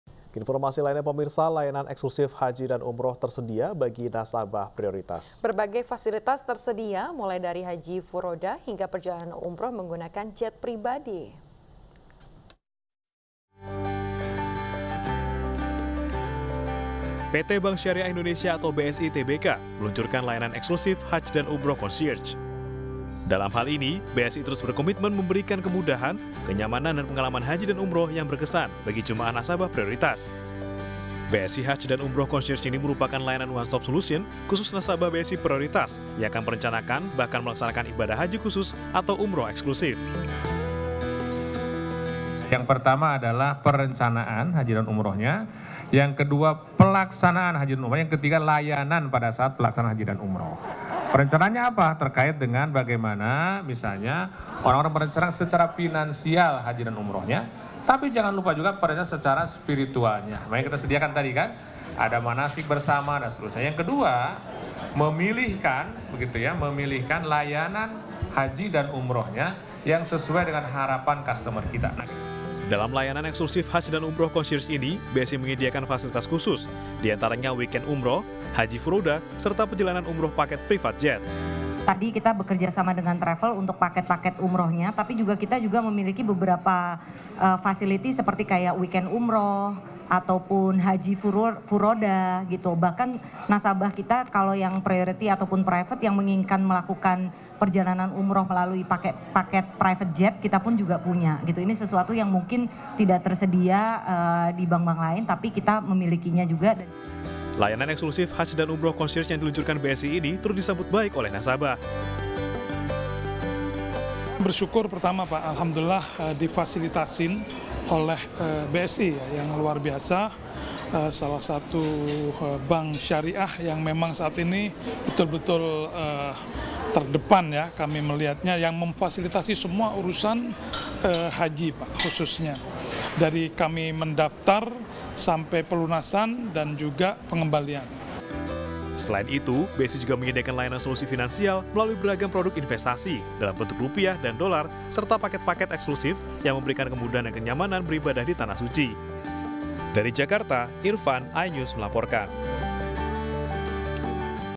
BSI Luncurkan Layanan Eksklusif Haji & Umrah Concierge untuk Nasabah Prioritas
PT Bank Syariah Indonesia Tbk (BRIS) meluncurkan layanan eksklusif Hajj & Umrah Concierge yang merupakan layanan dan fasilitas yang diberikan Bank BSI para nasabah prioritasnya. Direktur Sales and Distribution Anton Sukarna mengatakan layanan BSI Hajj & Umrah Concierge merupakan layanan one stop solution khusus untuk nasabah BSI Prioritas yang akan merencanakan atau melaksanakan ibadah haji khusus atau umrah eksklusif dengan berbagai keistimewaan lainnya.